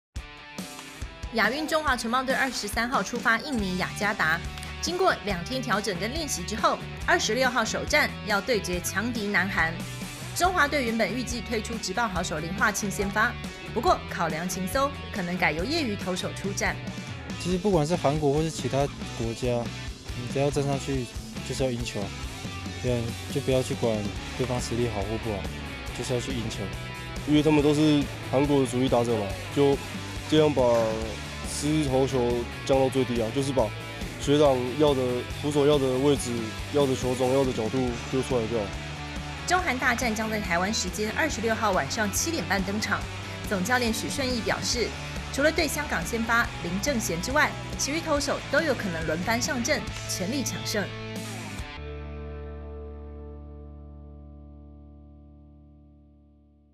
亞運》棒球首戰強碰南韓 中華隊全力搶勝
中華棒球隊明天晚上7:30就要對決韓國啦！一起幫他們加油吧！